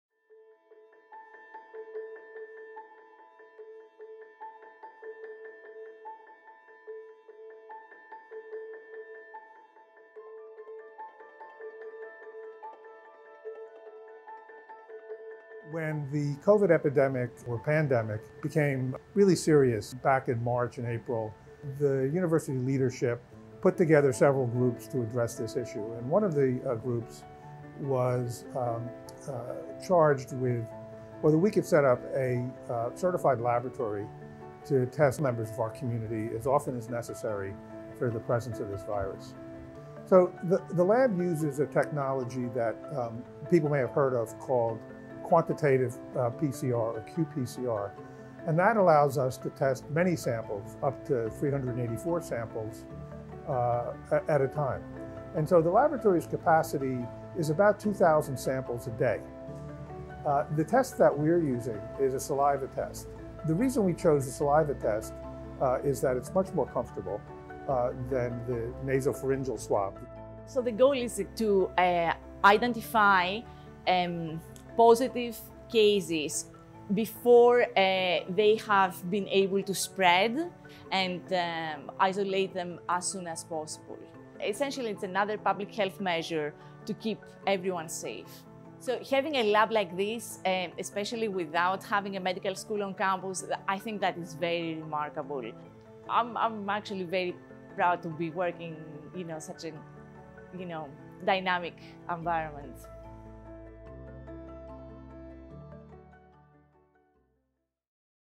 Princeton University COVID-19 Testing Laboratory
Princeton University is opening a new COVID-19 testing laboratory to support its comprehensive testing program for students, faculty, staff and researchers approved to be on campus. The in-house clinical laboratory will facilitate increased testing capabilities with faster results. The lab’s opening is an important step as the University considers how to safely increase density on campus and support the return of residential education.